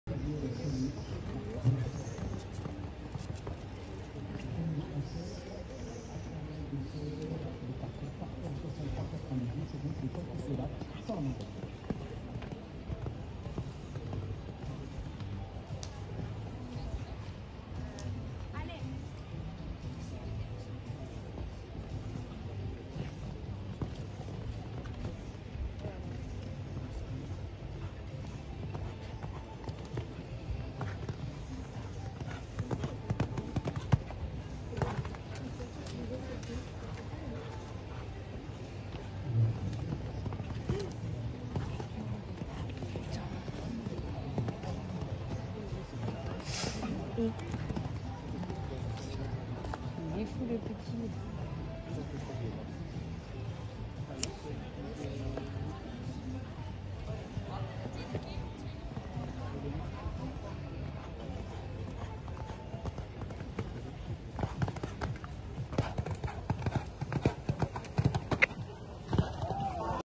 Vital Chance*de la Roque - 1.50m CSI 5* Grimaud - 27/08/2020
Vital Chance*de la Roque et Marc Dilasser - 1.50m CSI 5* Grimaud - 27/08/2020